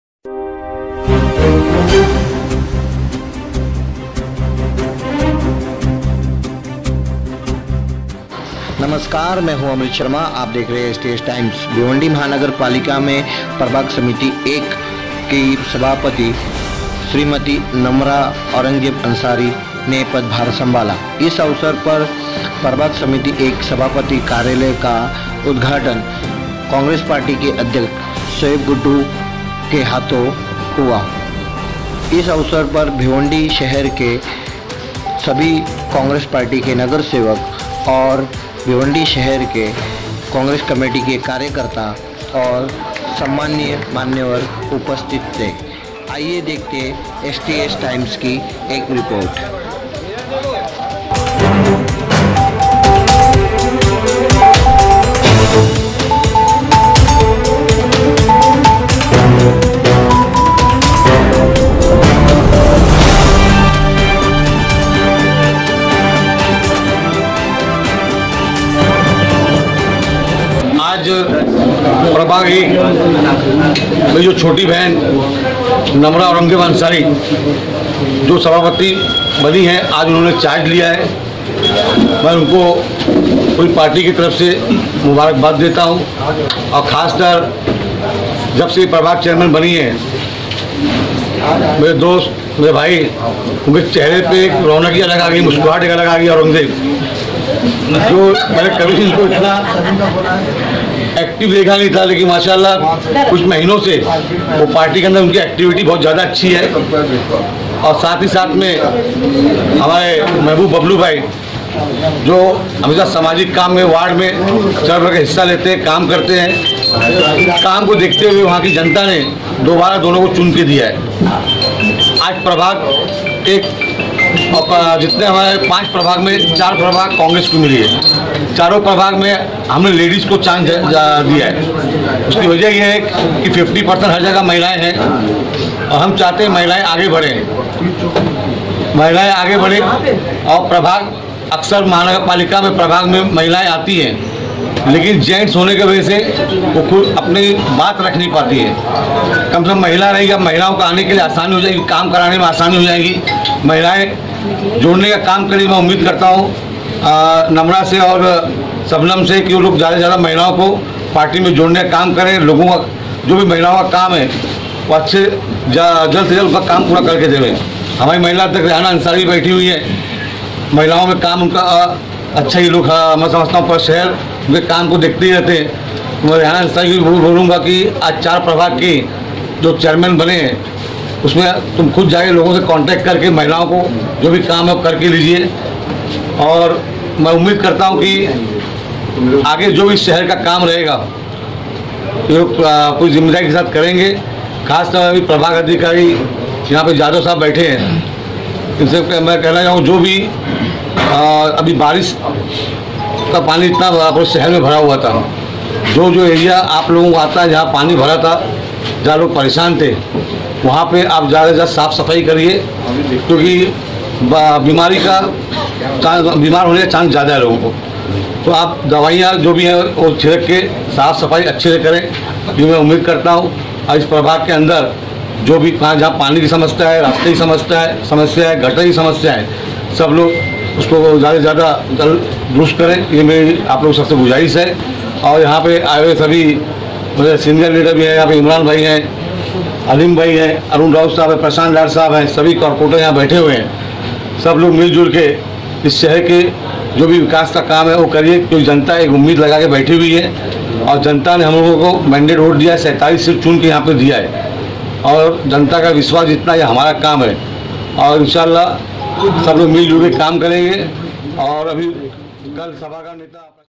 भिवंडी मनपा प्रभाक समिति एक की सभापति का पदभार संभाला श्रीमती नमरा औरंगजेब अंसारी
STS Times
रिपोर्ट by
Amrut sharma